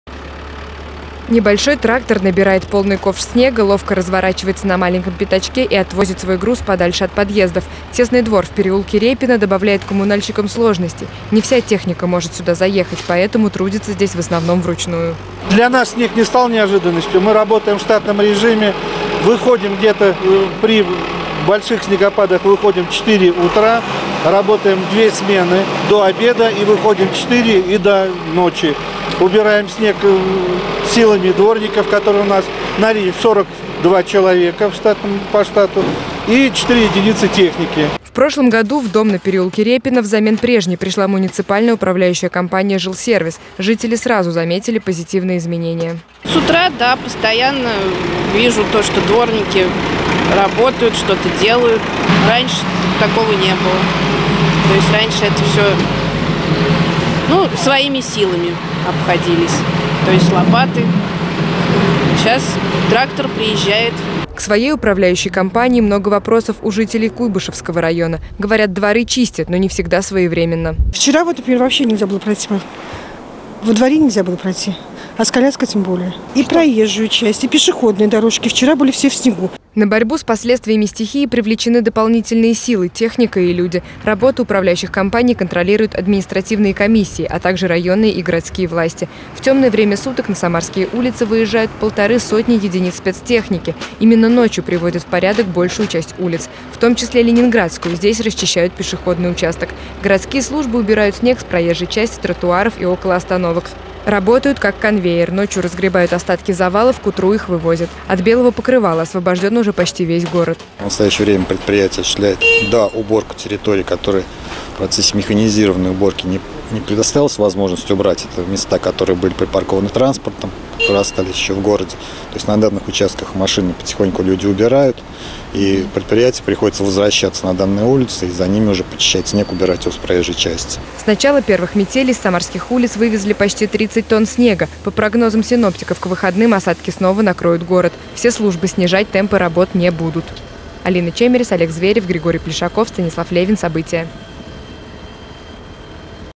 И днем, и ночью. Коммунальщики освобождают Самару от снежного плена
Что им снег.  Из-за сильных метелей самарские коммунальщики работают практически круглосуточно. Дороги, тротуары и дворы от последствий стихии очищают сотни человек и единиц спецтехники.  С начала первых метелей уже вывезли десятки тонн снега.